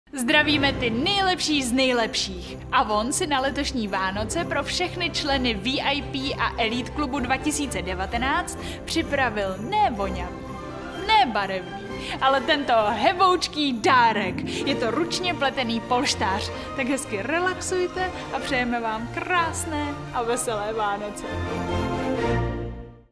Vánoční dárek pro roční členy AVON VIP a ELITE klubu
Vánoční dárek pro ty nejlepší z nejlepších! 🎄💗 Tento rok si AVON
pro roční členy AVON VIP a ELITE klubu připravil úžasný měkký dárek - ručně pletený polštář! Líbí se vám? 😍